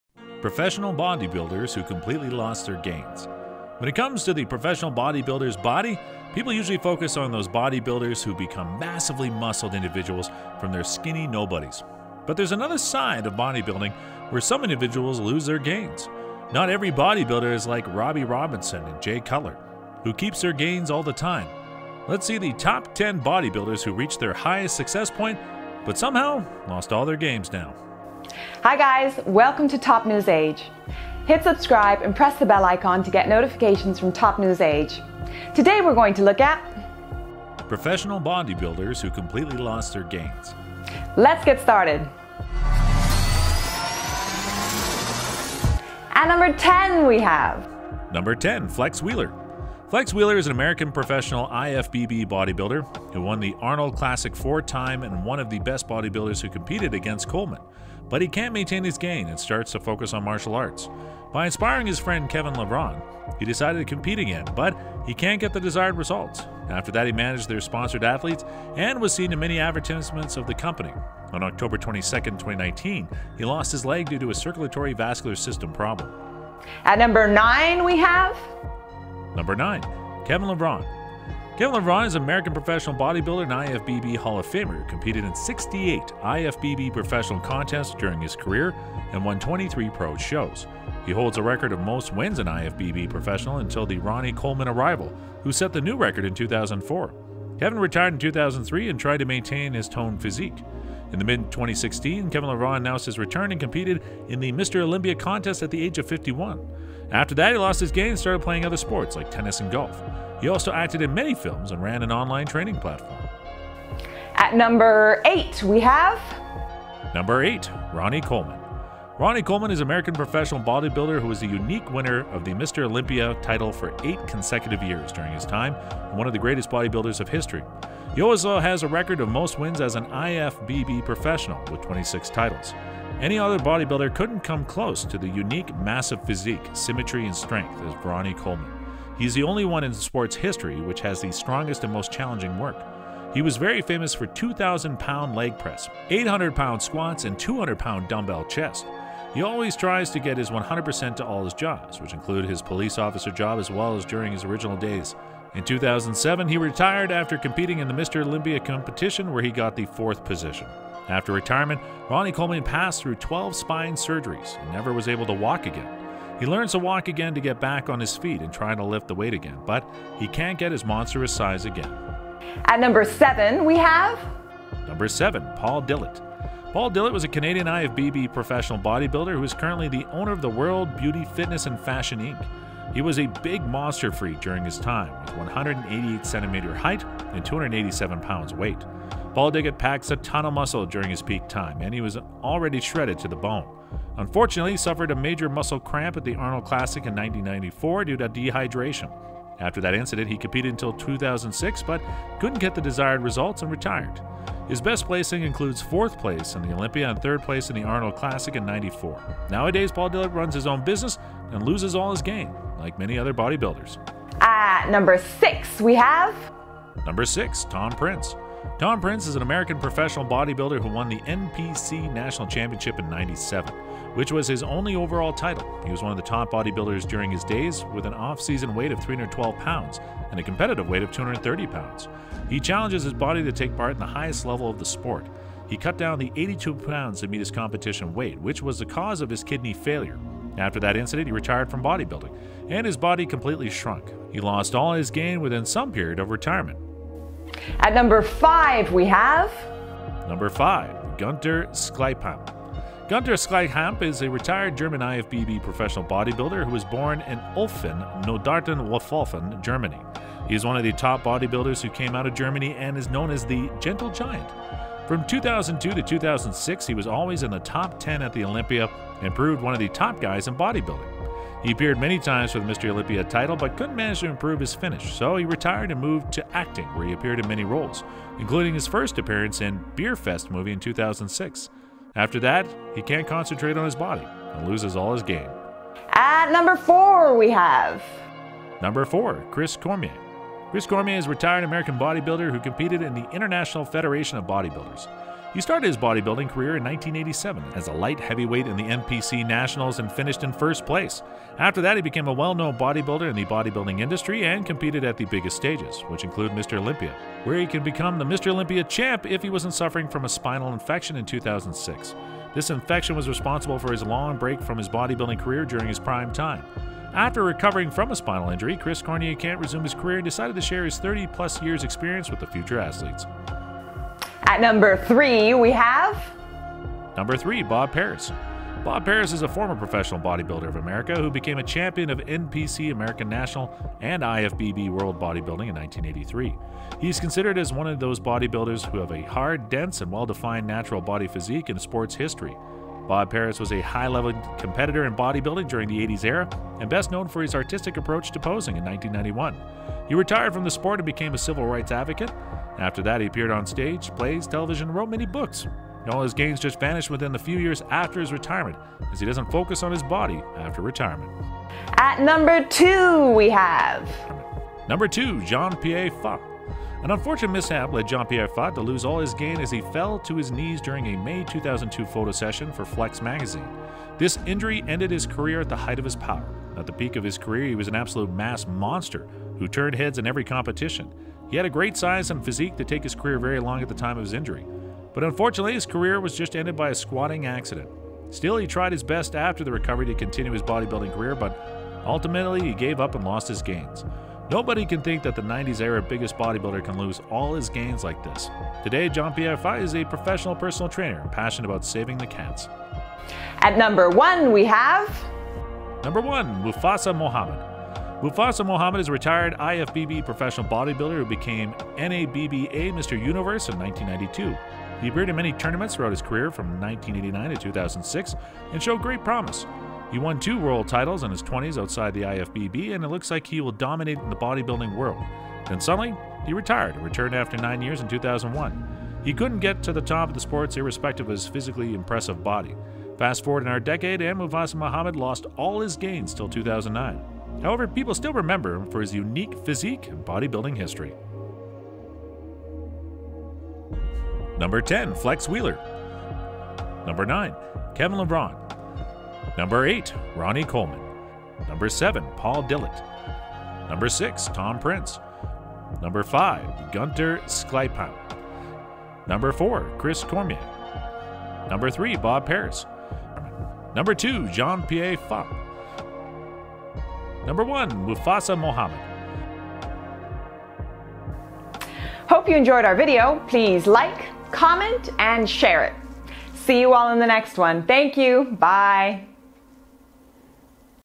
Top 10 Bodybuilders who lost their gains by TopNewsage
Top 10 professional bodybuilders who lost their gains due to various reasons. Some of them lost their gains due to injuries, while others couldn't maintain their physique after retirement. The list includes famous bodybuilders such as Flex Wheeler, Kevin Levrone, Ronnie Coleman, and Paul Dillett. The text also highlights the achievements and careers of each bodybuilder before they lost their gains.